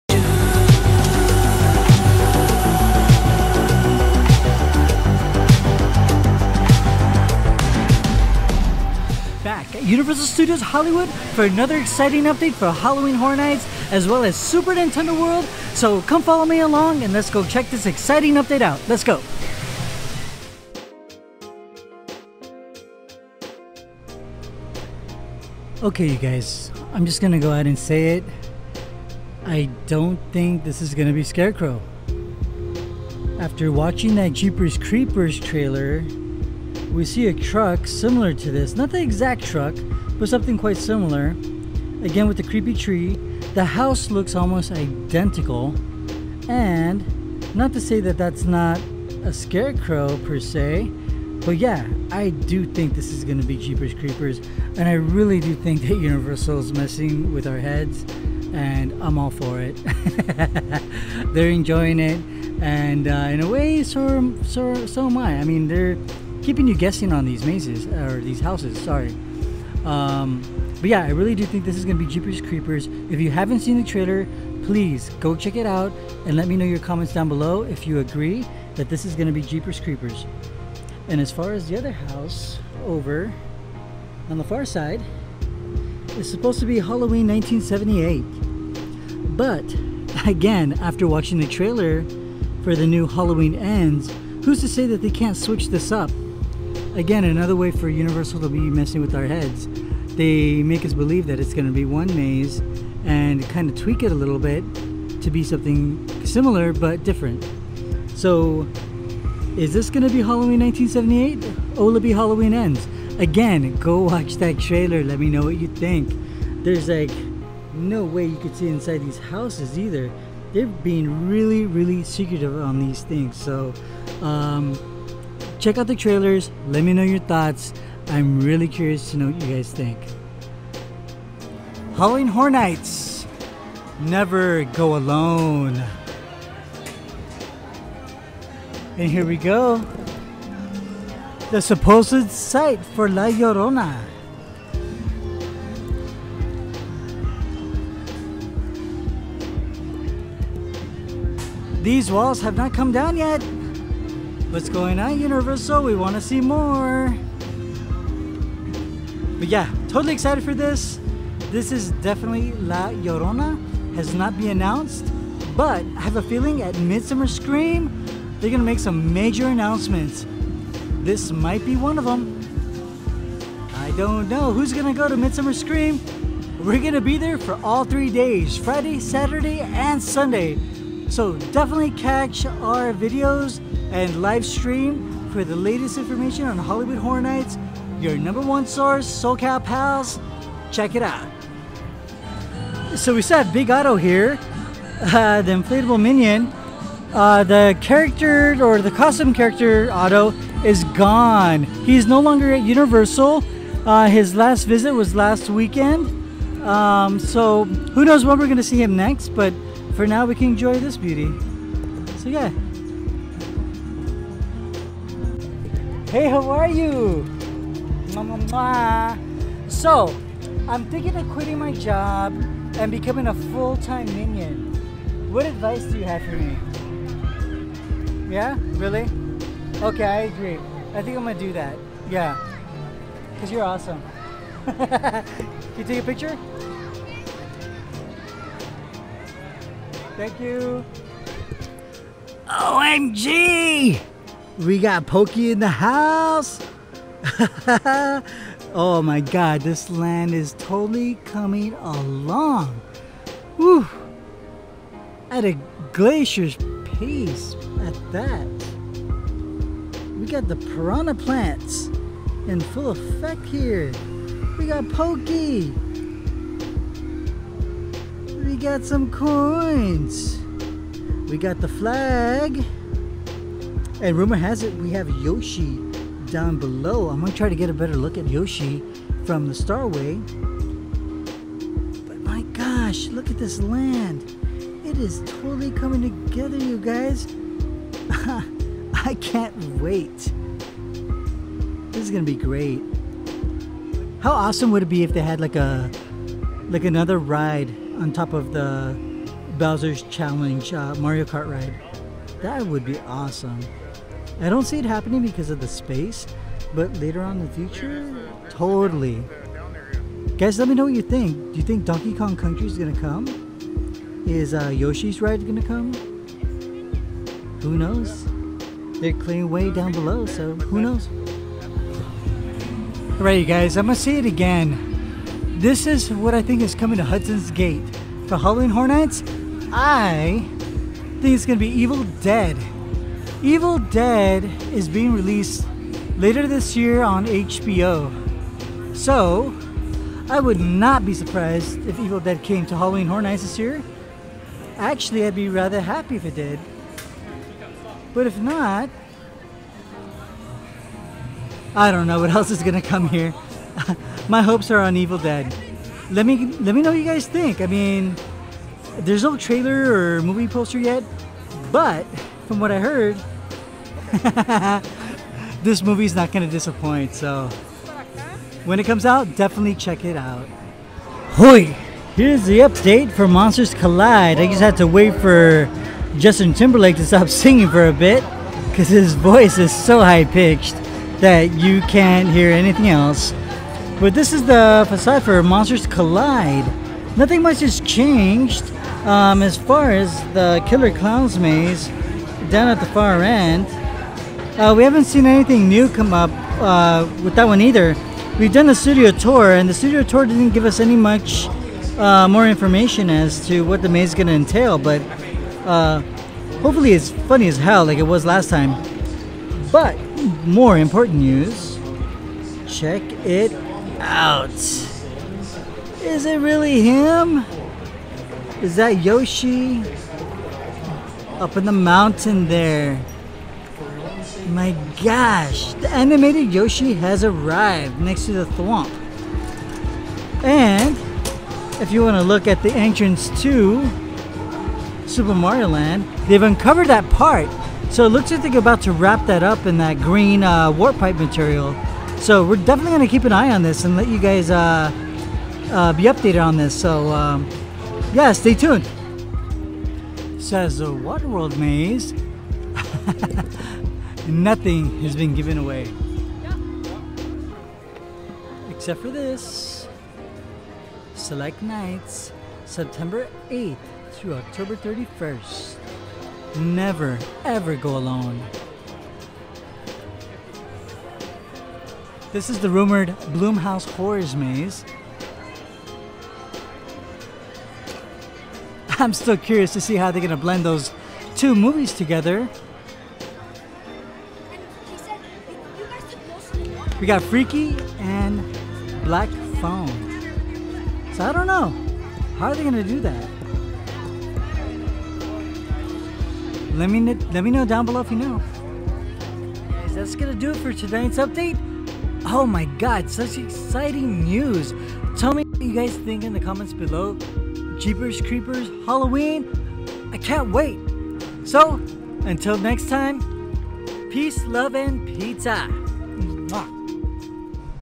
Halloween Horror Nights MAJOR Speculation Update! | NEW Super Nintendo World Construction reveals!
Universal Studios Hollywood updates for Halloween Horror Nights and Super Nintendo World. Yoshi has arrived!!!